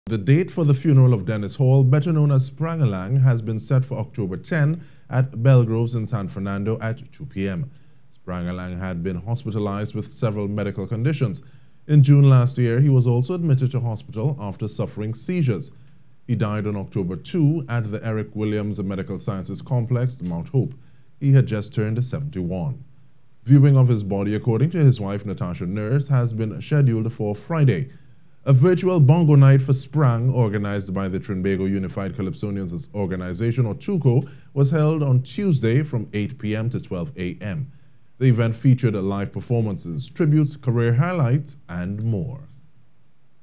Funeral For Dennis 'Sprangalang' Hall Set For Saturday
The date for the funeral of Dennis Hall, better known as "Sprangalang," has been set for October 10th at Belgroves in San Fernando at 2pm.

Sprangalang had been hospitalised with several medical conditions. In June last year he was also admitted to hospital after suffering seizures.

He died on October 2nd at the Eric Williams Medical Sciences Complex, Mt Hope. He had just turned 71.

Viewing of his body according to his wife Natasha Nurse has been scheduled for Friday.

A virtual Bongo Night Fuh Sprang, organized by the Trinbago Unified Calypsonians' Organisation (TUCO, was held on Tuesday from 8pm to 12am.  The event featured live performances, tributes, career highlights and more.